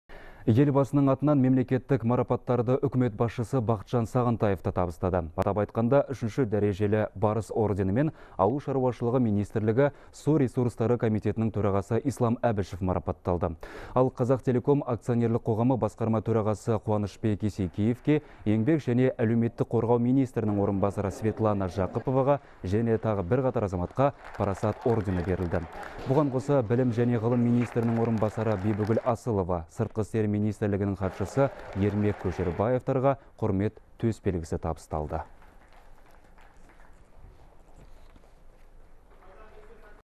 Б.Сағынтаев бірқатар әріптестерін мемлекеттік наградалармен марапаттады
Үкіметте Тәуелсіздік күніне орай марапаттау рәсімі өтті, деп хабарлайды «Хабар 24».

Премьер-министр Бақытжан Сағынтаев Президент атынан бірқатар әріптестерін мемлекеттік наградалармен марапаттады.

Атап айтқанда, Ауыл шаруашылығы министрлігі Су ресурстары комитетінің төрағасы Ислам Әбішевке III дәрежелі «Барыс» ордені, «Қазақтелекомның» басқарма төрағасы Қуанышбек Есекеевке, Еңбек және халықты әлеуметтік қорғау министрінің орынбасары Светлана Жақыповаға және тағы бірқатар азаматқа «Парасат» ордені берілді. Сондай-ақ Білім және ғылым министрінің орынбасары Бибігүл Асылова, Сыртқы істер министрлігінің хатшысы Ермек Көшербаевтарға «Құрмет» төсбелгісі табысталды.